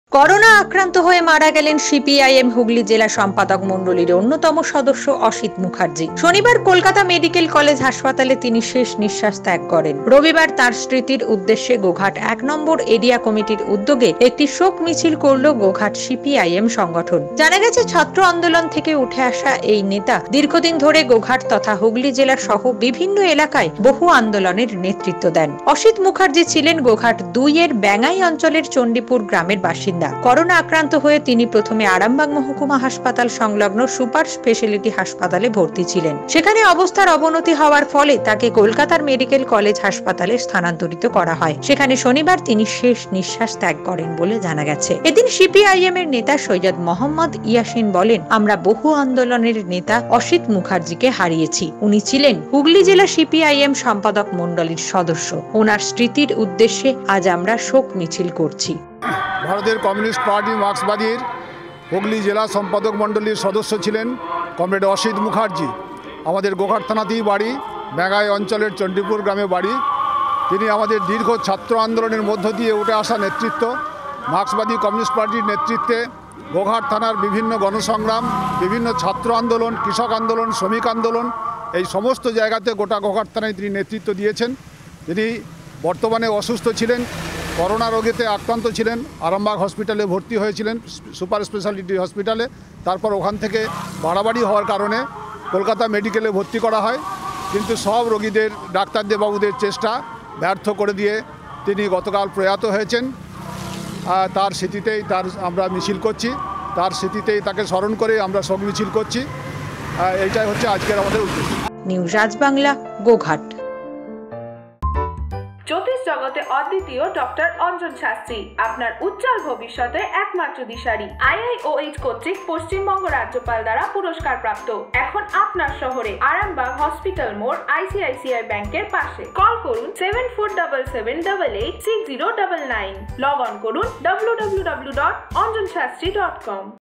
অসিত মুখার্জির স্মরণে শোক মিছিল
শনিবার কলকাতা মেডিকেল কলেজ হাসপাতালে মারা গেলেন বামনেতা অসিত মুখার্জি। তাঁর স্মরণে রবিবার শোক মিছিল করল গোঘাট সিপিআইএম